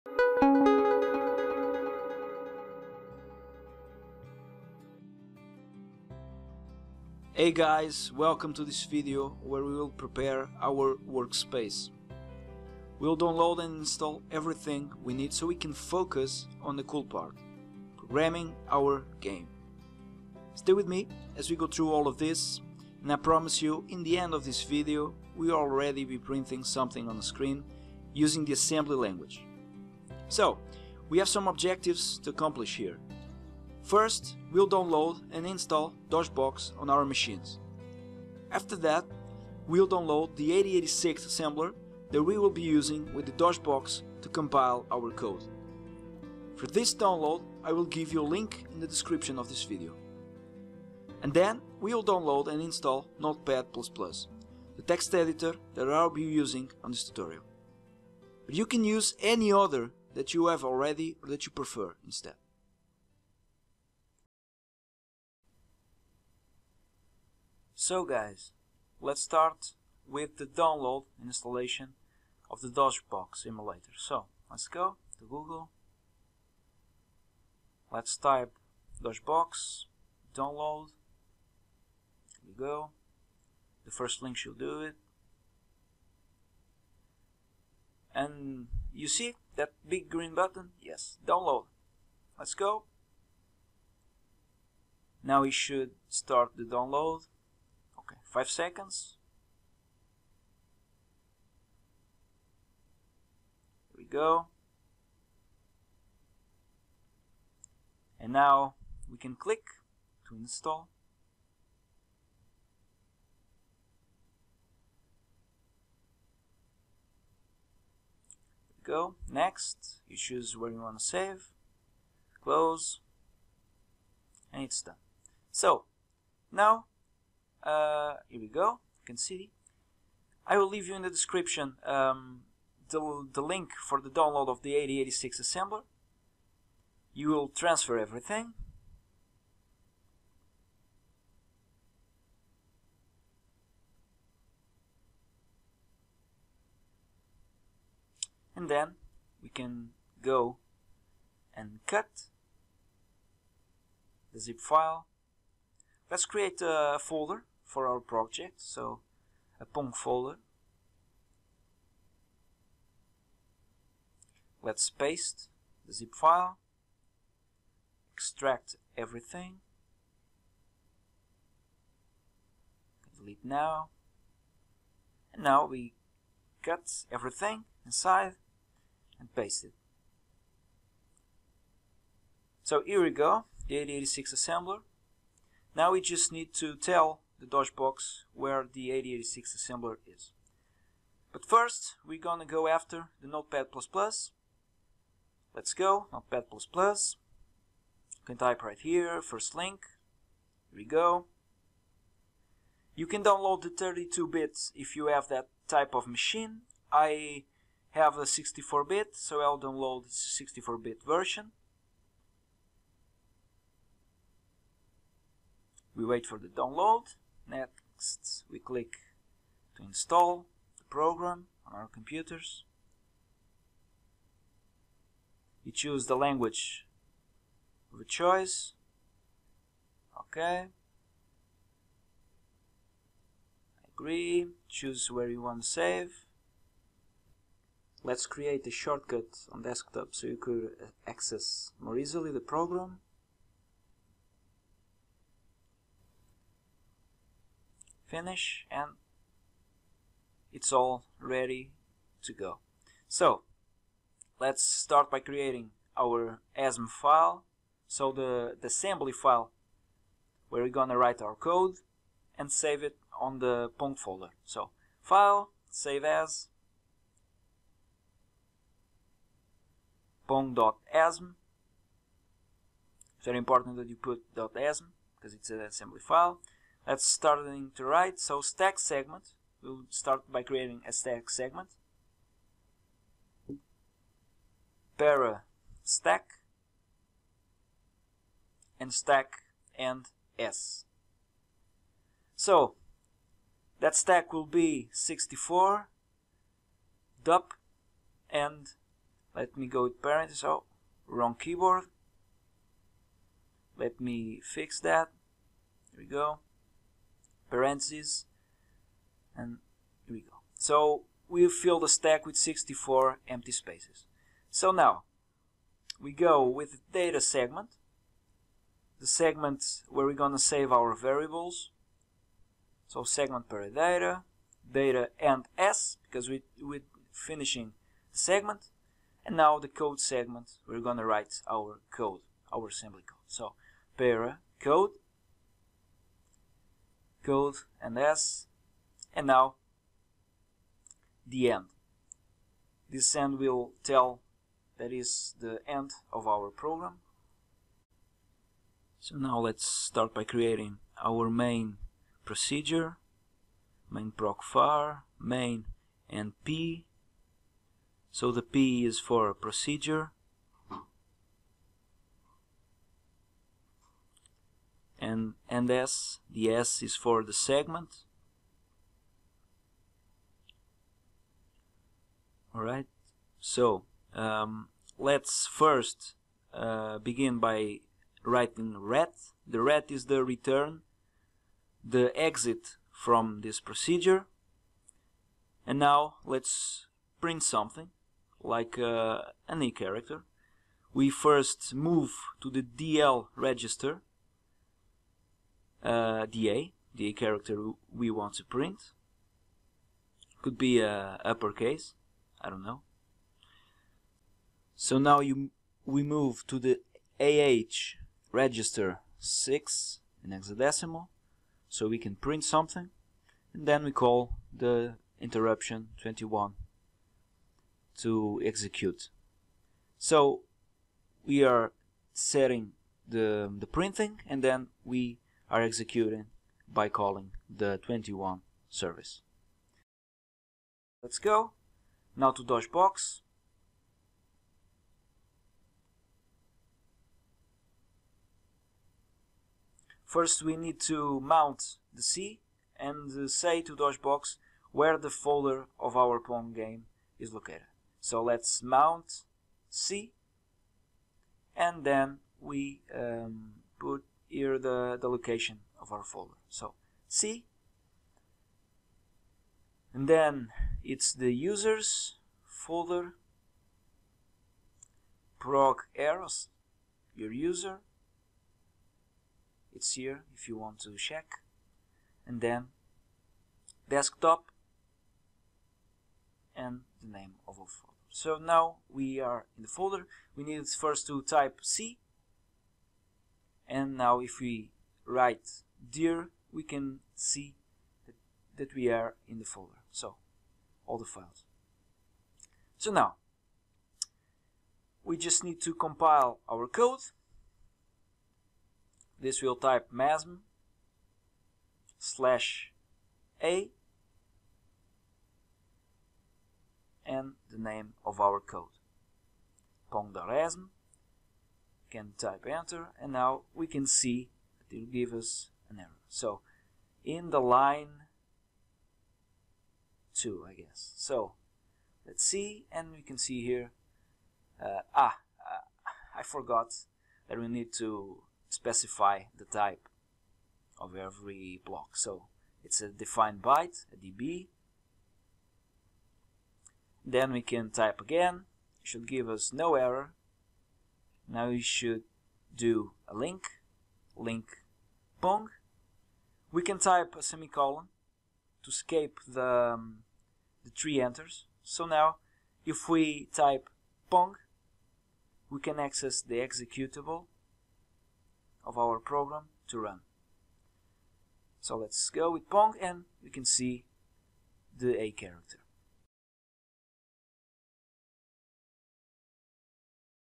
[MASM] 8086 Assembly - Pong - 02 Preparing Our Workspace
In this video we will download and install everything we need to compile, execute and write our Assembly program.

At the end of the video we will begin our Assembly journey by printing to the screen a character.